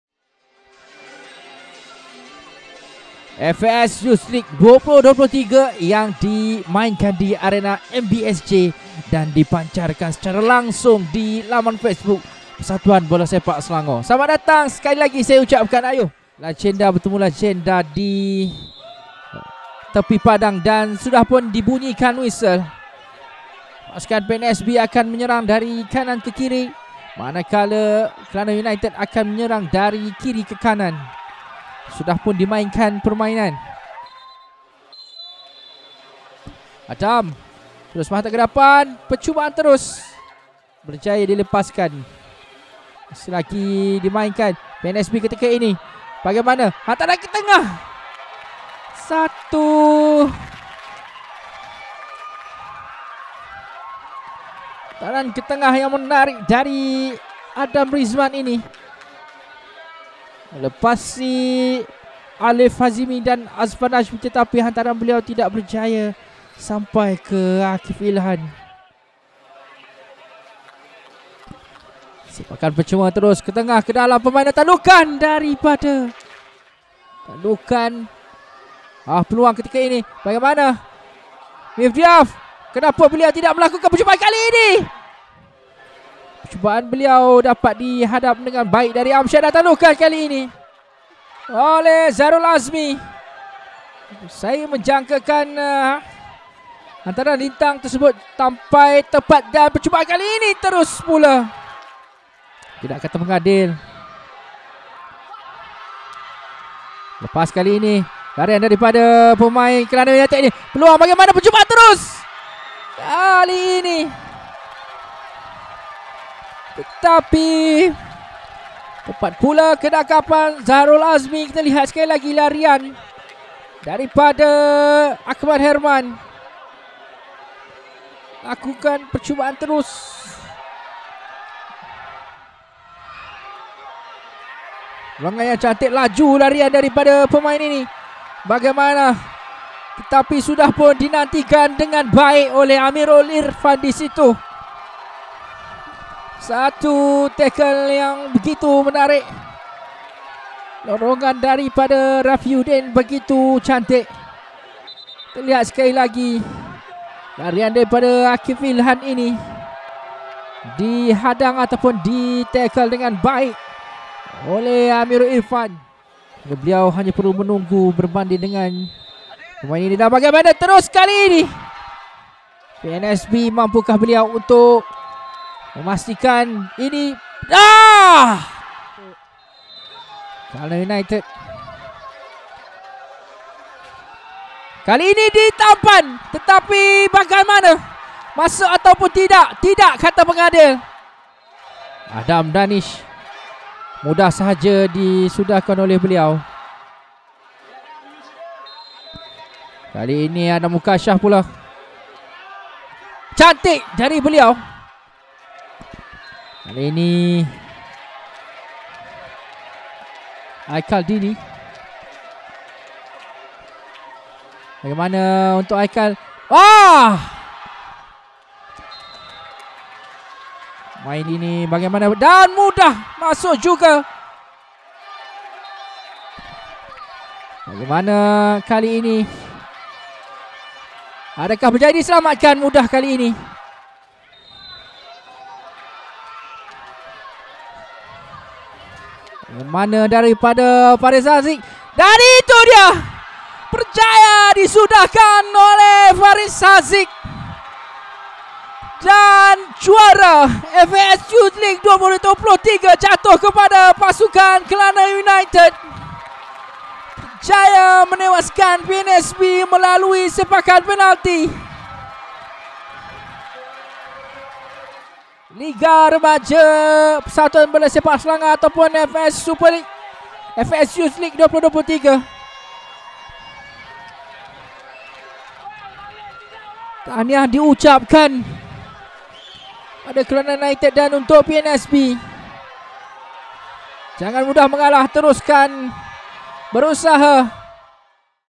LIGA REMAJA FAS U19 2023: KELANA UNITED 0 (4-2) 0 PNSB
Sorotan perlawanan akhir Liga Remaja FAS U19 2023 diantara Kelana United dan juga PNSB.